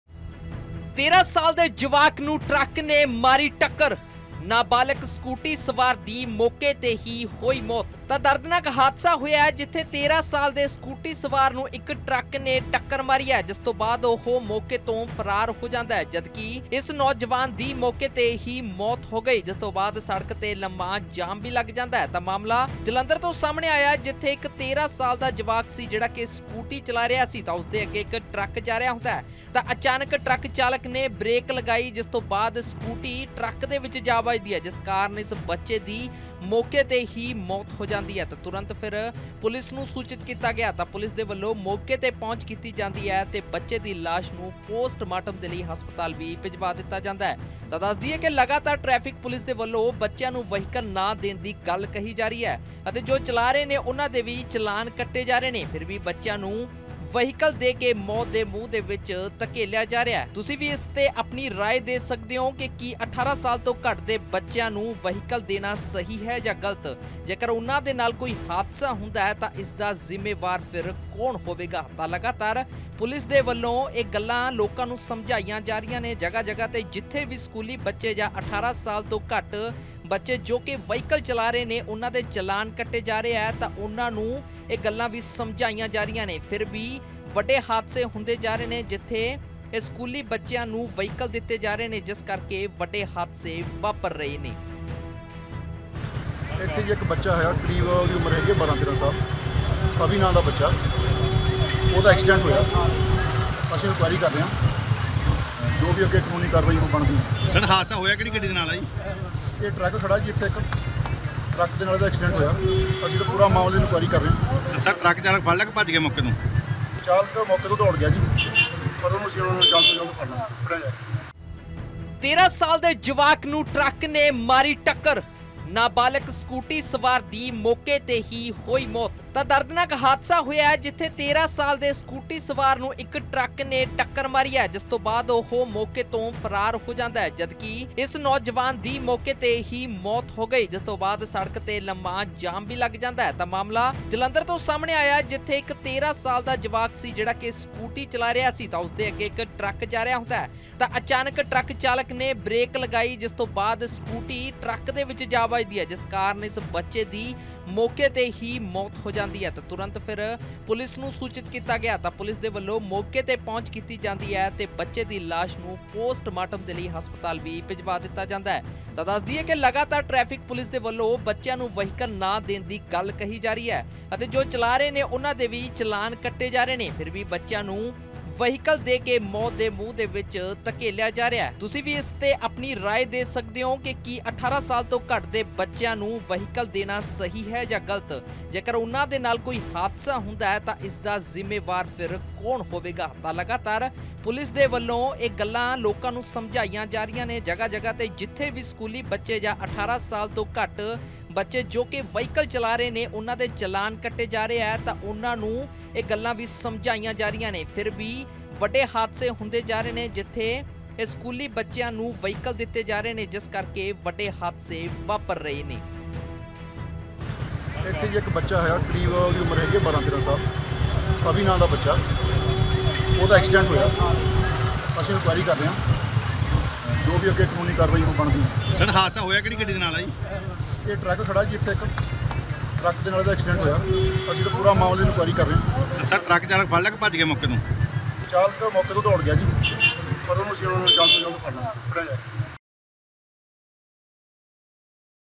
Jalandhar ’ਚ ਟਰੱਕ ਨੇ ਅਚਾਨਕ ਮਾਰੀ ਬ੍ਰੇਕ, ਹੇਠਾਂ ਆਇਆ ਨਬਾਲਗ ਸਕੂਟੀ ਸਵਾਰ... ਮੌਕੇ ’ਤੇ ਮੌਤ
ਟਰੱਕ ਨੇ ਅਚਾਨਕ ਮਾਰੀ ਬ੍ਰੇਕ, ਹੇਠਾਂ ਆਇਆ ਨਬਾਲਗ ਸਕੂਟੀ ਸਵਾਰ... ਮੌਕੇ ’ਤੇ ਮੌਤ